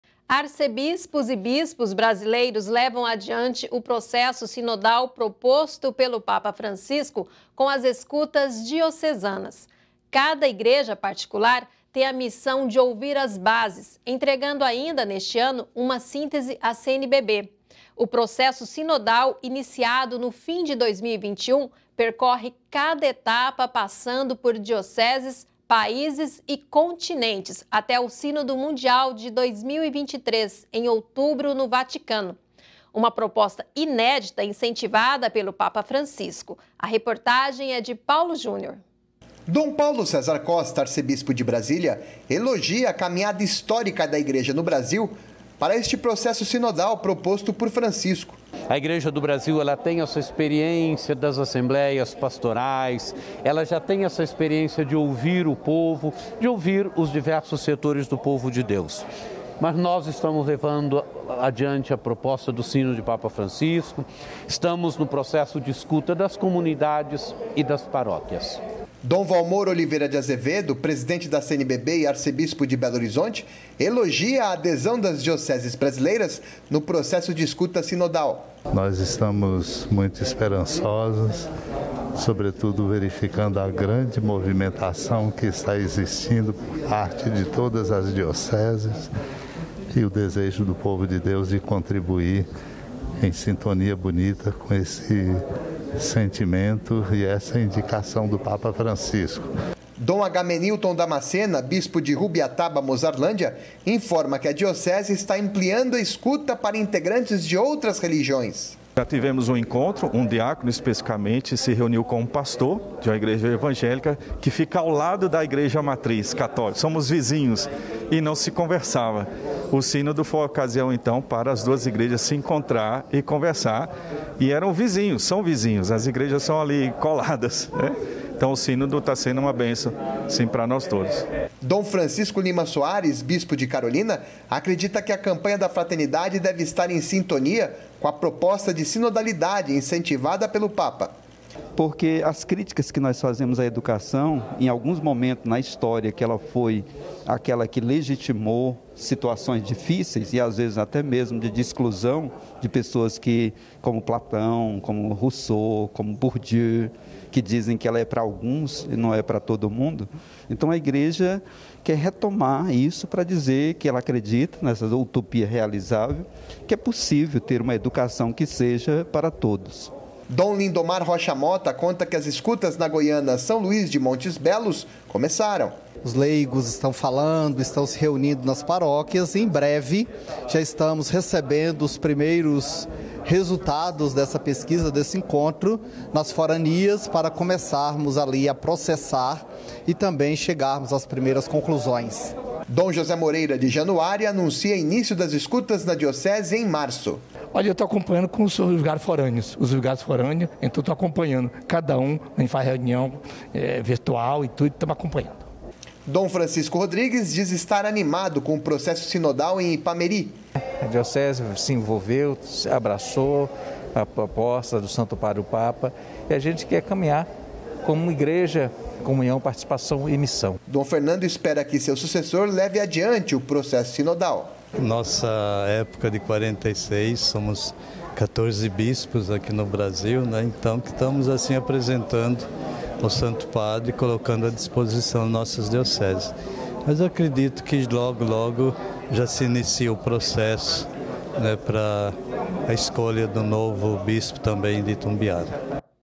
Episcopado trabalha pelo êxito do processo de escuta do sínodo dos bispos – JCTV – 24/02/22
Arcebispos e bispos brasileiros levam adiante o processo sinodal proposto pelo papa Francisco com as escutas diocesanas. Cada igreja particular tem a missão de ouvir as bases, entregando ainda neste ano uma síntese à CNBB. A reportagem é de Paulo Júnior.